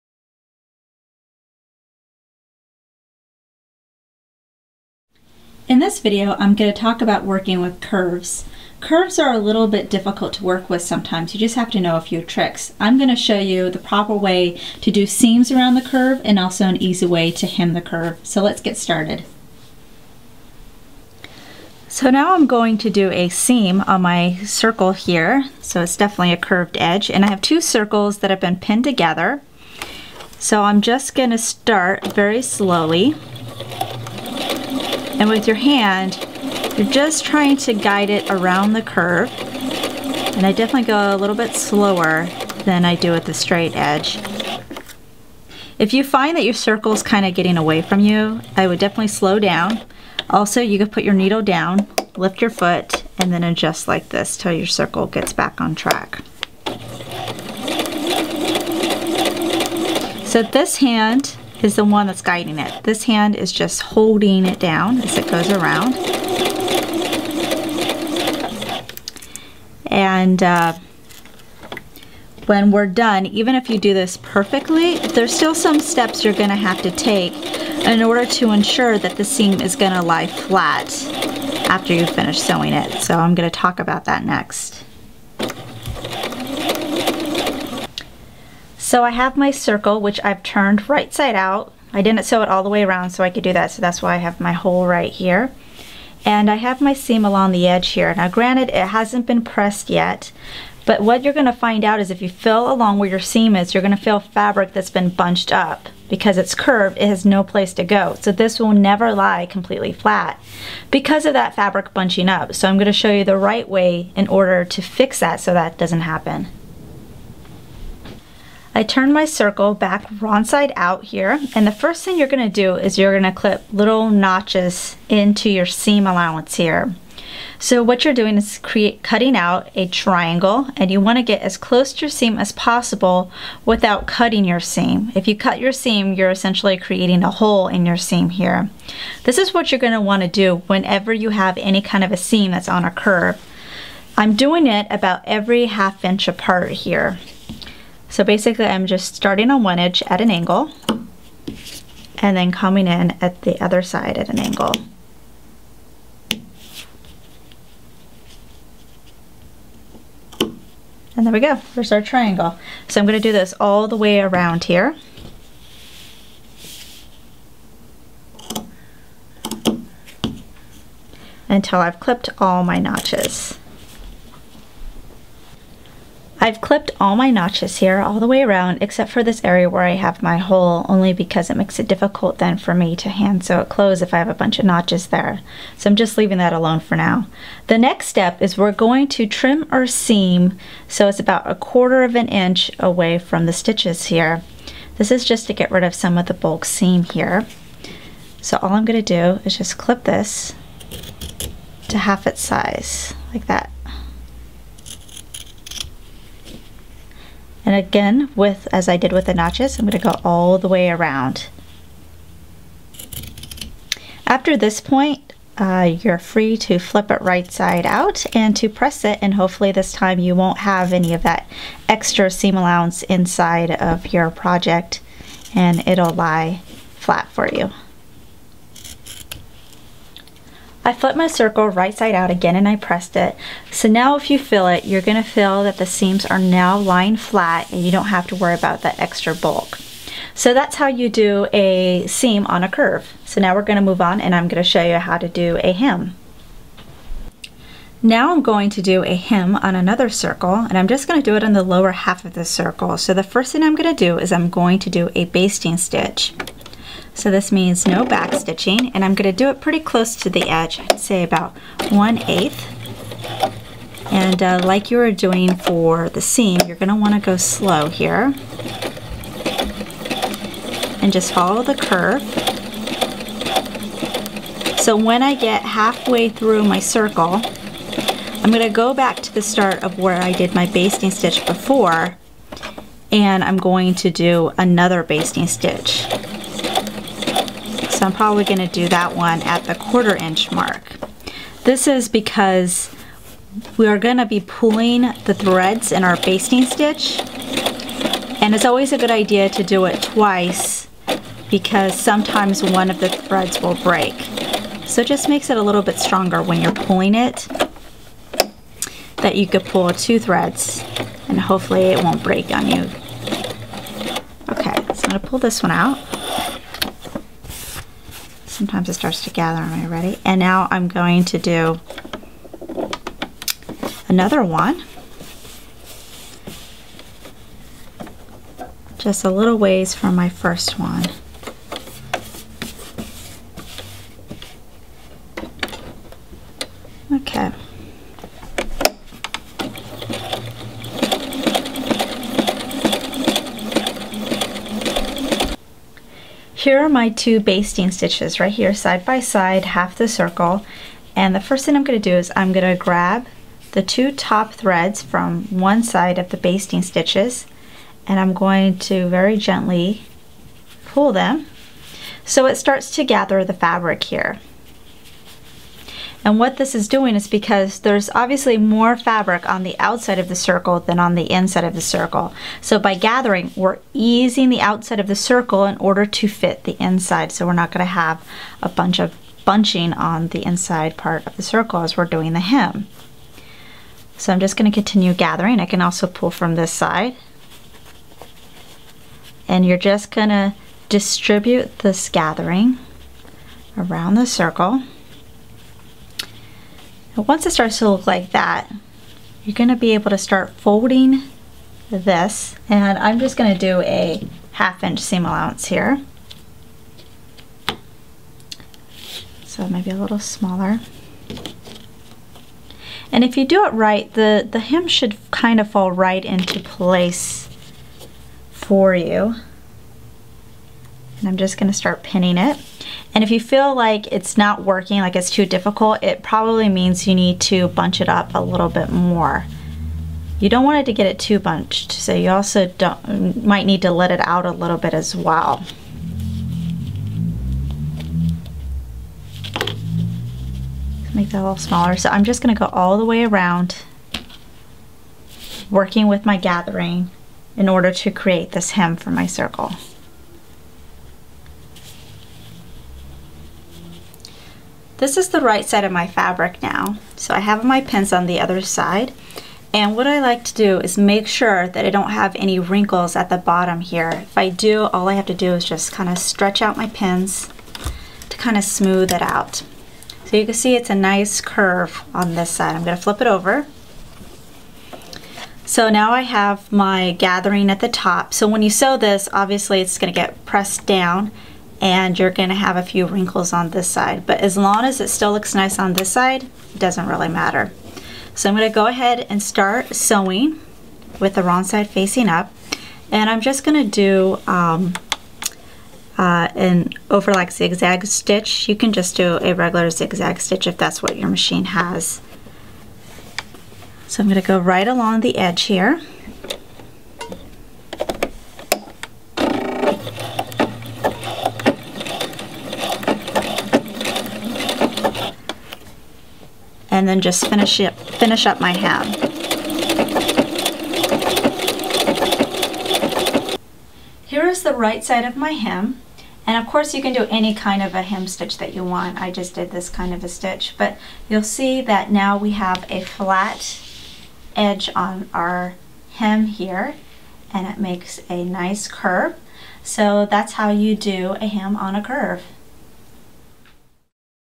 How To Sew With Curves
Break away from only sewing straight lines and learn the fundamentals of sewing with curves.